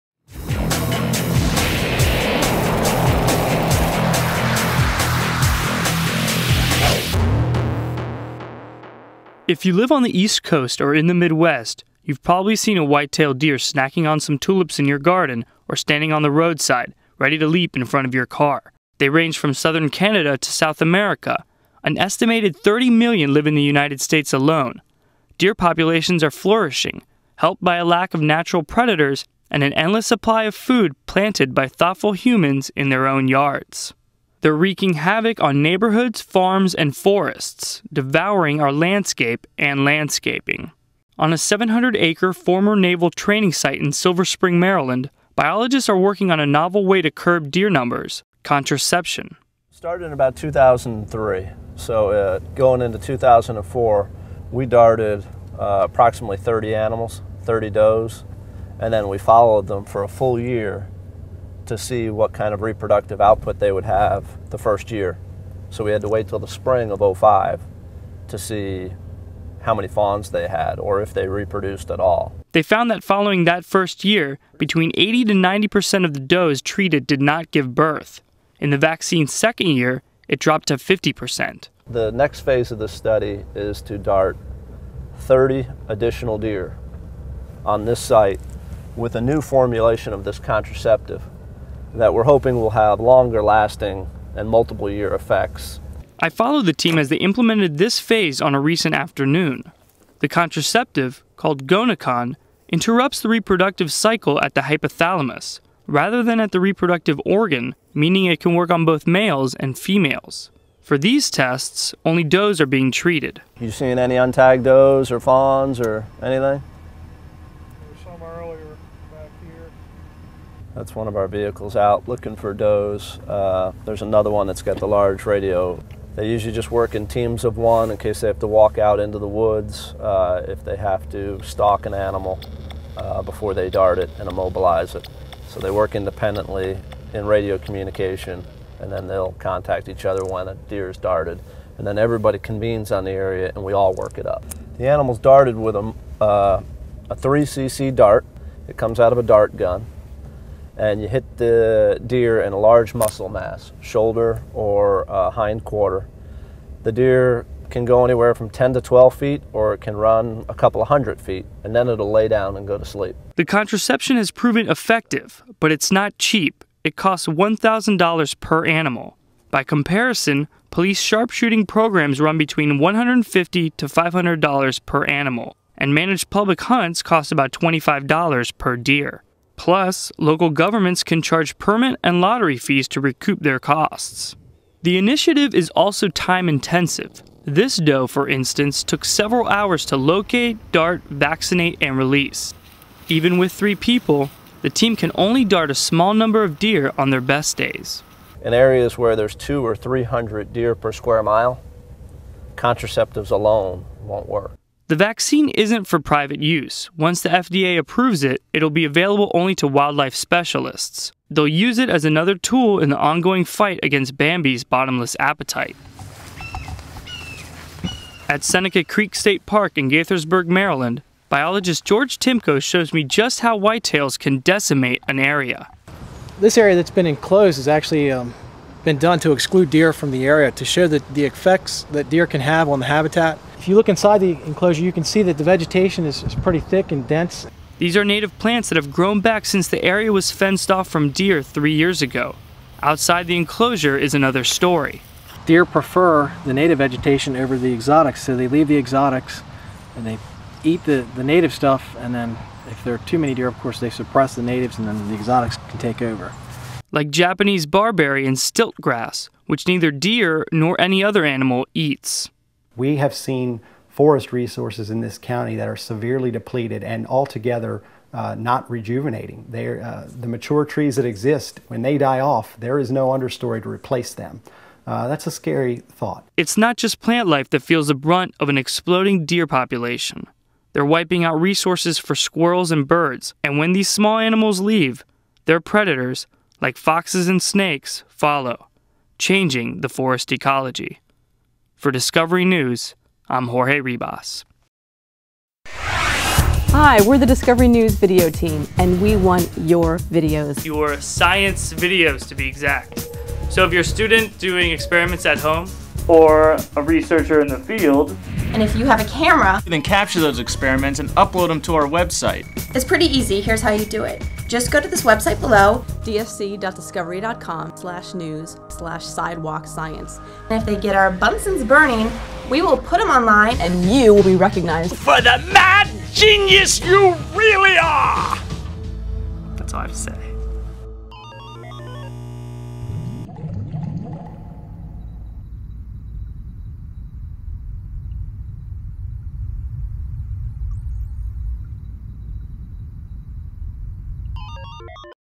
Birth Control for Bambi
Researchers are testing a new contraceptive vaccine as a means to control a white-tailed deer population that is devastating yards, farms and forests. Jorge Ribas tags along.